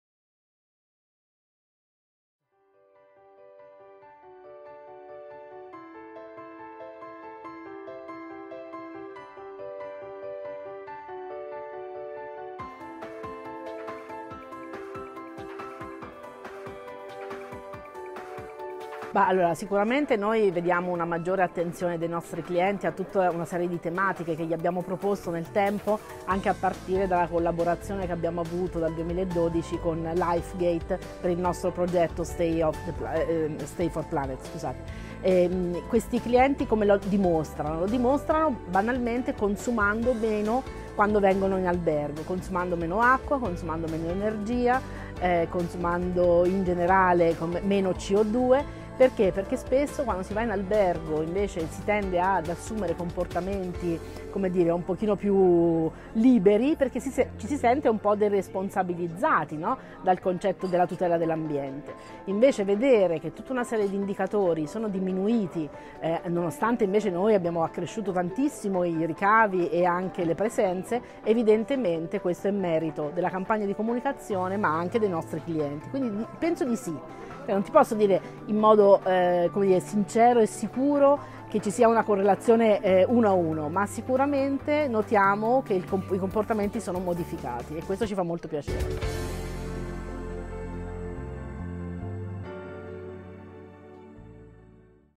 Giovanna Manzi, Best Western Italia, all'Osservatorio LifeGate 2018
“Noi vediamo una sempre maggiore attenzione dei nostri clienti verso una serie di tematiche relative alla sostenibilità che abbiamo proposto loro nel tempo". — Giovanna Manzi, dg Best Western Italia

I DATI 2018 DEL QUARTO OSSERVATORIO NAZIONALE SULLO STILE DI VITA SOSTENIBILE SI POSSONO SCARICARE QUI:

Intervista a cura della redazione LifeGate. Riprese e montaggio: Luigi Zanni.